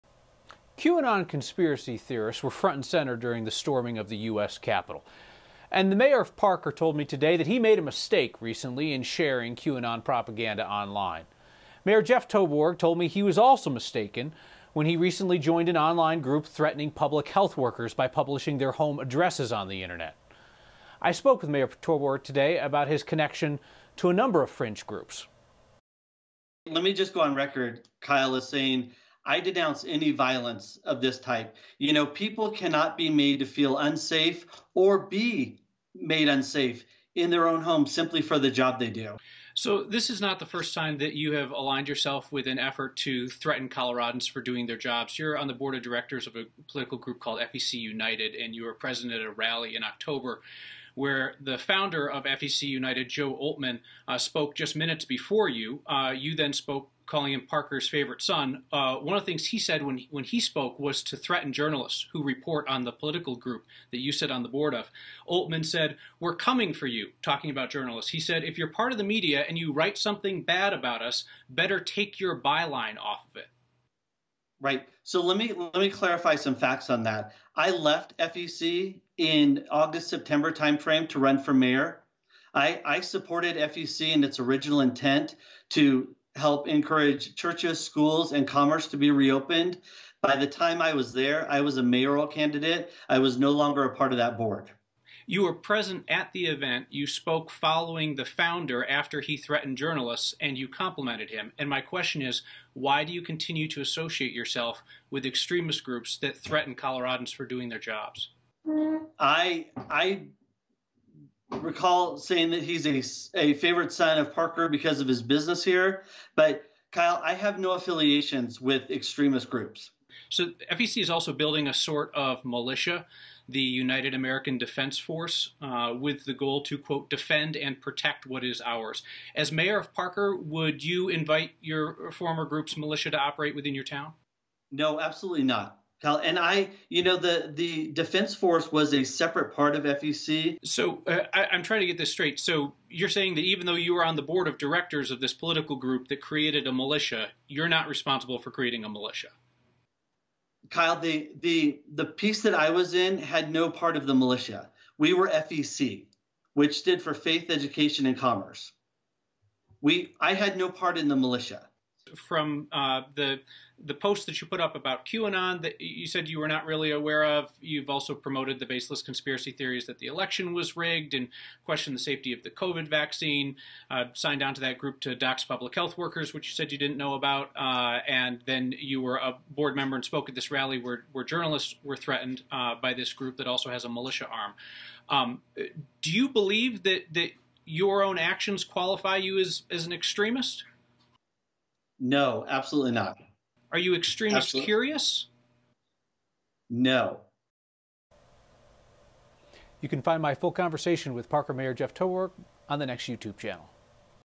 Parker mayor who shared QAnon post says he didn't know the meaning behind it
Mayor Jeff Toborg said he was also mistaken when he recently joined an online group threatening public health workers by publishing their home addresses online.